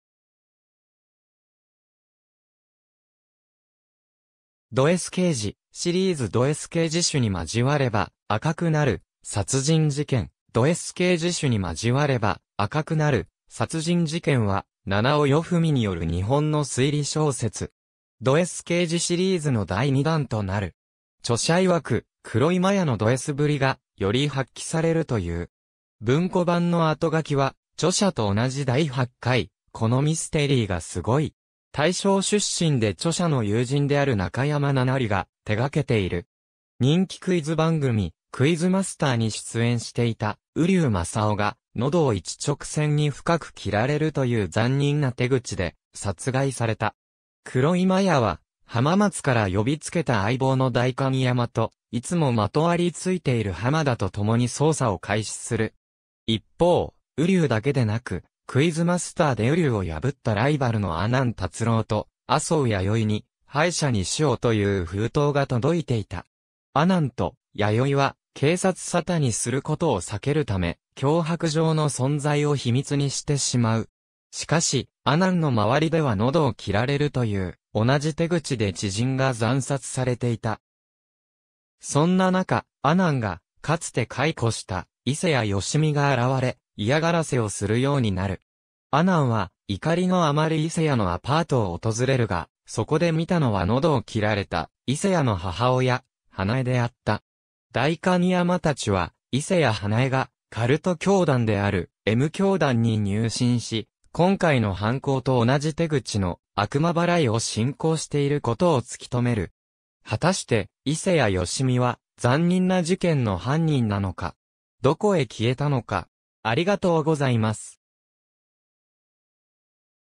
ドS刑事 朱に交われば赤くなる殺人事件
ドS刑事シリーズ 」 ドS刑事 朱に交われば赤くなる殺人事件 『ドS刑事 朱に交われば赤くなる殺人事件』（ドエスでか しゅにまじわればあかくなるさつじんじけん）は、七尾与史による日本の推理小説。
ドS刑事シリーズの第2弾となる。
著者いわく、黒井マヤのドSぶりがより発揮されるという。
文庫版のあとがきは著者と同じ第8回『このミステリーがすごい!』大賞出身で著者の友人である中山七里が手がけている。
人気クイズ番組「クイズマスター」に出演していた瓜生正夫が、喉を一直線に深く切られるという残忍な手口で殺害された。
黒井マヤは浜松から呼びつけた相棒の代官山といつもまとわりついている浜田とともに捜査を開始する。
一方、瓜生だけでなく、「クイズマスター」で瓜生を破ったライバルの阿南達郎と麻生弥生に「「敗者に死を」」という封筒が届いていた。
阿南と弥生は警察沙汰にすることを避けるため、脅迫状の存在を秘密にしてしまう。
しかし、阿南の周りでは喉を切られるという同じ手口で知人が惨殺されていた。
そんな中、阿南がかつて解雇した伊勢谷良美が現れ、嫌がらせをするようになる。
阿南は怒りのあまり伊勢谷のアパートを訪れるが、そこで見たのは喉を切られた伊勢谷の母親・花枝であった。
代官山達は伊勢谷花枝がカルト教団である「M教団」に入信し、今回の犯行と同じ手口の「悪魔払い」を信仰していることを突き止める。
果たして伊勢谷良美は残忍な事件の犯人なのか。
どこへ消えたのか。